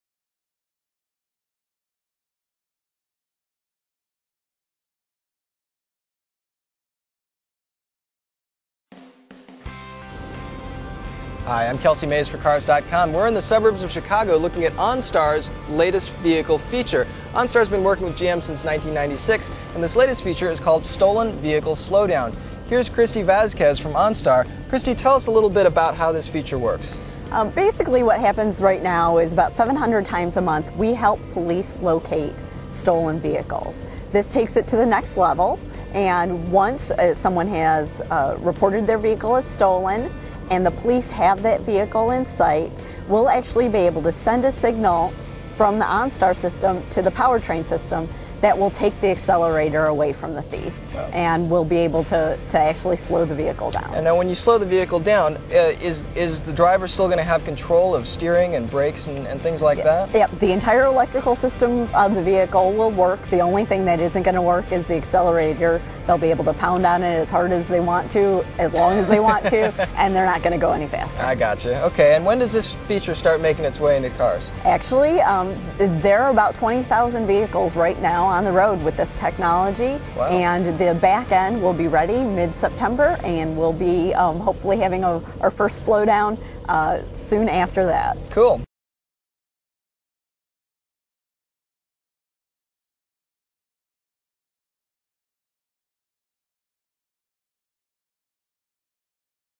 What Would Big Brother Do?
Big Brother or Big Help? OnStar 2009 can stop moving cars via satellite

General Motors and its partner, OnStar, plan to introduce a new feature to GM vehicles in 2009: the ability for police to remotely stop a moving car if it has been reported as stolen. While the move adds another weapon against grand theft auto, it also opens doors that are cause for concern.

"But one concern would be whether the police could slow down any car with OnStar capabilities whenever they want—after all, no one wants to be driving along and suddenly have the car forced to a (slow) stop for any old reason. Indeed, there are number of troubling ways in which the new OnStar tech could be misused and abused. But GM says that OnStar customers will have the ability to opt out of the Stolen Vehicle Slowdown feature at any time if they should so choose, although the company says that its research shows that 95 percent of its subscribers want the service for their vehicles. "

Can personal privacy survive the digital revolution?

Sprint Nextel, for example, provided the government with GPS locations of its subscribers via their cell-phone signals 8 million times between September 2008 and October 2009. As Soghoian writes, telecom and Internet providers all have special departments, many open 24 hours per day, whose staff do nothing but respond to legal requests. Their entire purpose is to facilitate the disclosure of their customers records to law enforcement and intelligence agencies.

If Sprint Nextal is so willing to help the government what would Onstar do?